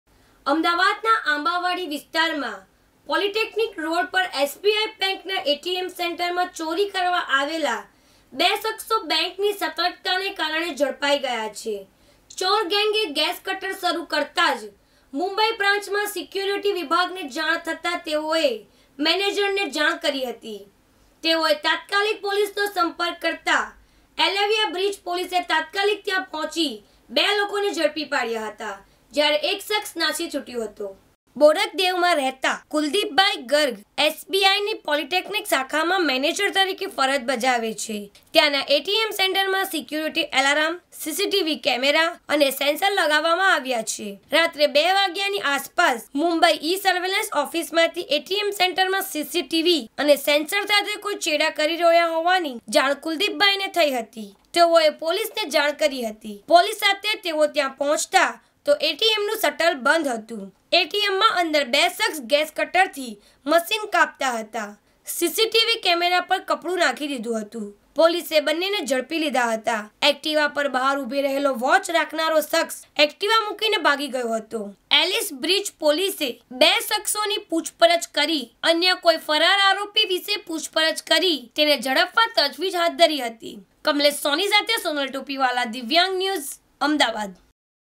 Ahemdabad l આંબાવાડીમાં SBI બેન્કના ATMમાં ચોરી કરવા આવેલા બે શખ્સ બેંકની સતર્કતાથી ઝડપાયા
Ahemdabad l આંબાવાડીમાં SBI બેન્કના ATMમાં ચોરી કરવા આવેલા બે શખ્સ બેંકની સતર્કતાથી ઝડપાયા l Divyang News